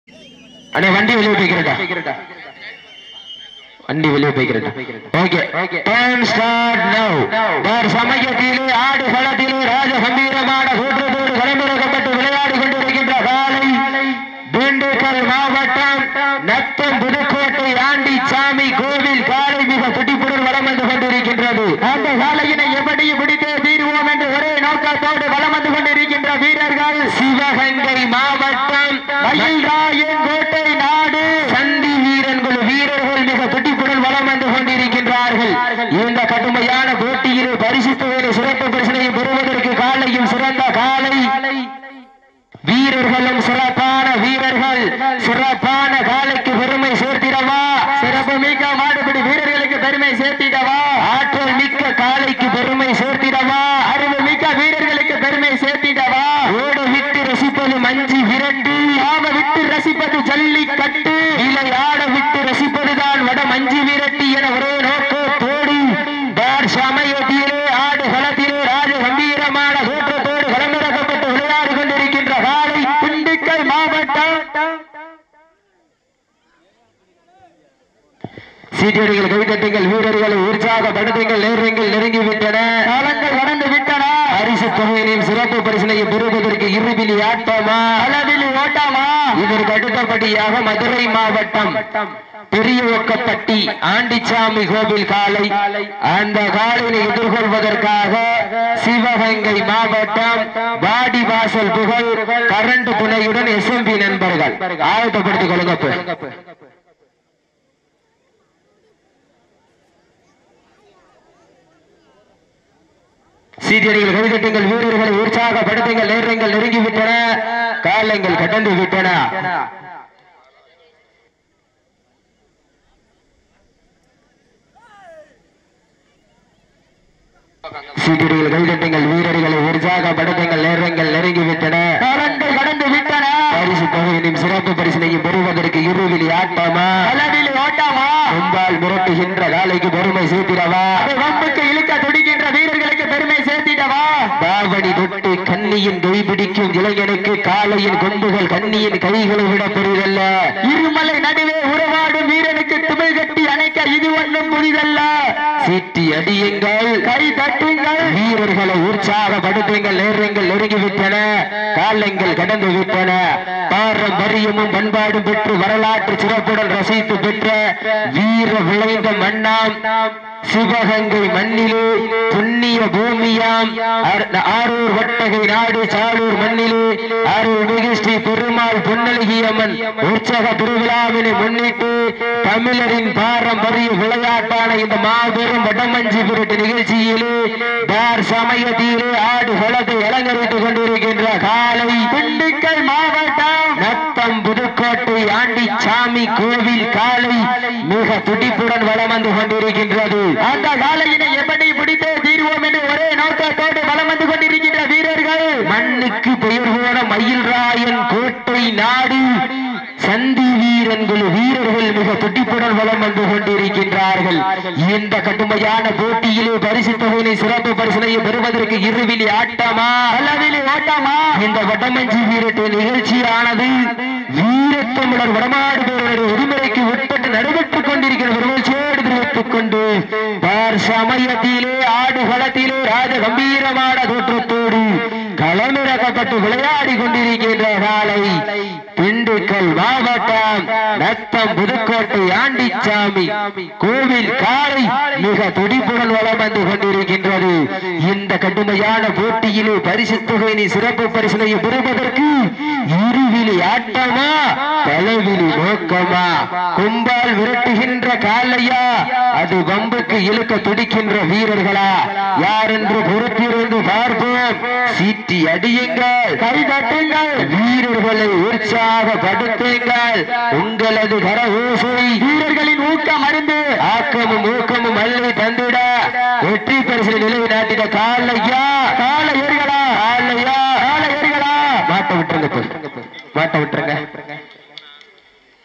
மயில்ராயன்கோட்டை நாடு சந்திவீரன் குழு Vs நத்தம் புதுக்கோட்டை ஆண்டிச்சாமி கோவில் காளை | PK Media
Please avoid any other methods of copying or duplicating the video, and help us support anti-piracy measures in any way you can. Thank you-PK Media